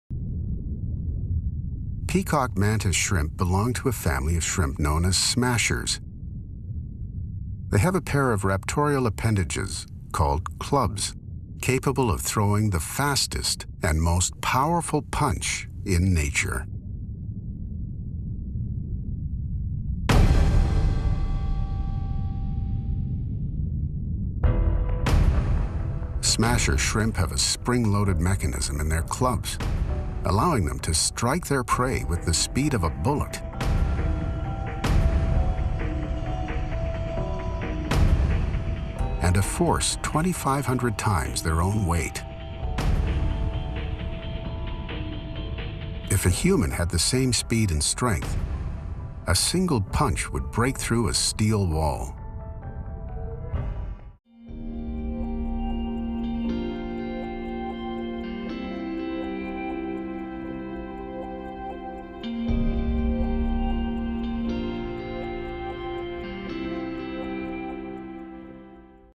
This Tiny Crustacean Has the Most Powerful Punch in the World
Peacock mantis shrimp possess a secret weapon: a devastatingly powerful punch that strikes prey with the speed of a bullet and a force 2,500 times the shrimp’s own weight.